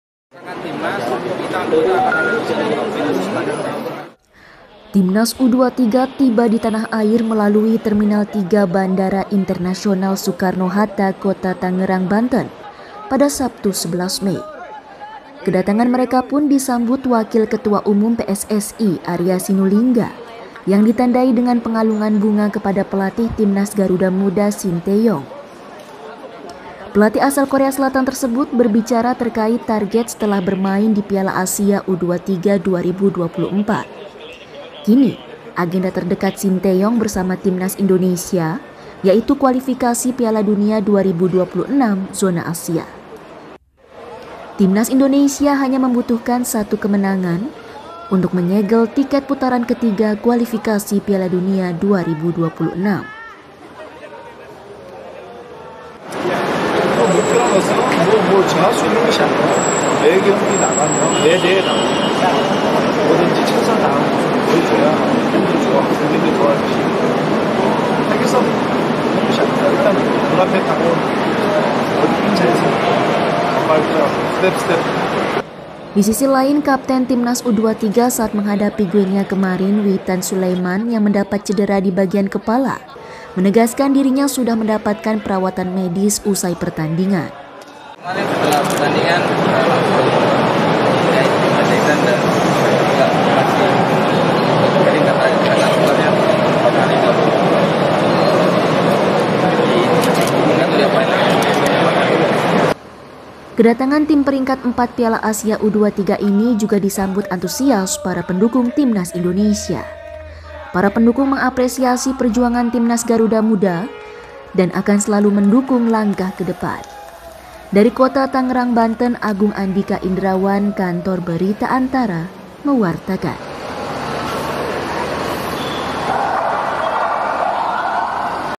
Tiba di Tanah Air, STY fokus persiapan kualifikasi Piala Dunia 2026
ANTARA - Pasukan Garuda muda atau Timnas U-23 Indonesia tiba di Terminal 3, Bandara Internasional Soekarno-Hatta, Kota Tangerang, Banten, Sabtu (11/5). Kedatangan mereka pun disambut perwakilan PSSI dengan pengalungan bunga. Pelatih Timnas U-23 Shin Tae-yong memastikan akan fokus mempersiapkan Timnas senior di dua sisa laga Kualifikasi Piala Dunia 2026. (Agung Andhika Indrawan/Yovita Amalia/Farah Khadija)